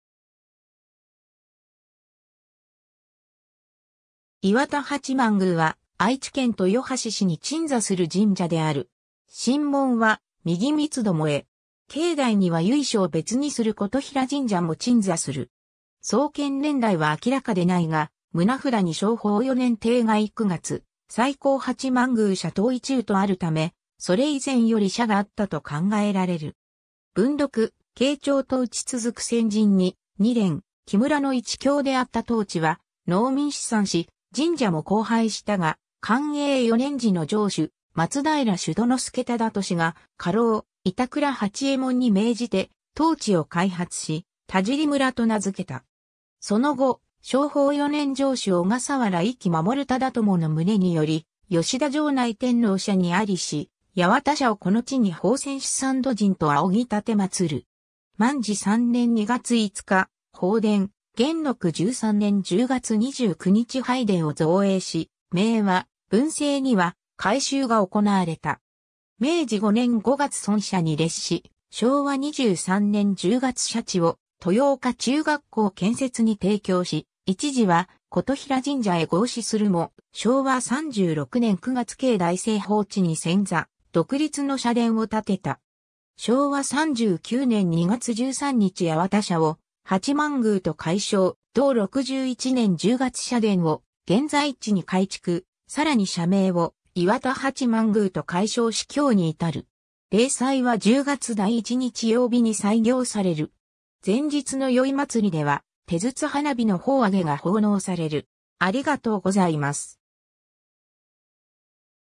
岩田八幡宮
岩田八幡宮（いわたはちまんぐう）は、愛知県豊橋市に鎮座する神社（八幡宮）である。
神紋は右三つ巴。
境内には由緒を別にする琴平神社も鎮座する。
 創建年代は明らかでないが、棟札に正保4年(1647年)丁亥9月「再興八幡宮社頭一宇」とあるため、それ以前より社があったと考えられる。
文禄・慶長(1593～1615)と打ち続く戦塵に、二連木村の一郷であった当地は農民四散し、神社も荒廃したが、寛永4年(1627年)時の城主松平主殿助忠利が家老板倉八右衛門に命じて当地を開発し田尻村と名付けた。
其の後、正保4年城主小笠原壱岐守忠知の旨により、吉田城内天王社にありし八幡社をこの地に奉遷し産土神と仰ぎ奉る。
 万治3年(1660年)2月5日宝殿、元禄13年(1700年)10月29日拝殿を造営し、明和(1768年)・文政(1818年)には改修が行われた。
明治5年5月村社に列し、昭和23年10月社地を豊岡中学校建設に提供し、一時は琴平神社へ合祀するも、昭和36年9月境内西方地(現護国神社)に遷座、独立の社殿を建てた。
昭和39年2月13日八幡社を八幡宮と改称、同61年10月社殿を現在地に改築、更に社名を岩田八幡宮と改称し今日に至る。
 例祭は10月第1日曜日に斎行される。
前日の宵祭では手筒花火の放揚が奉納される。